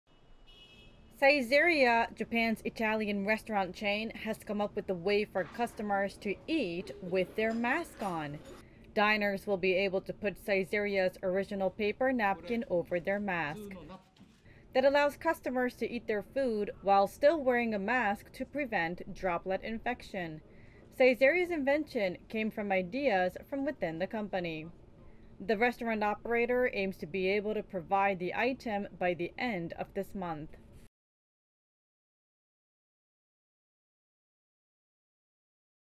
When the food comes, keep your mask on
Japan’s Italian restaurant chain Saizeriya invents a way for customers to dine out while still wearing their mask.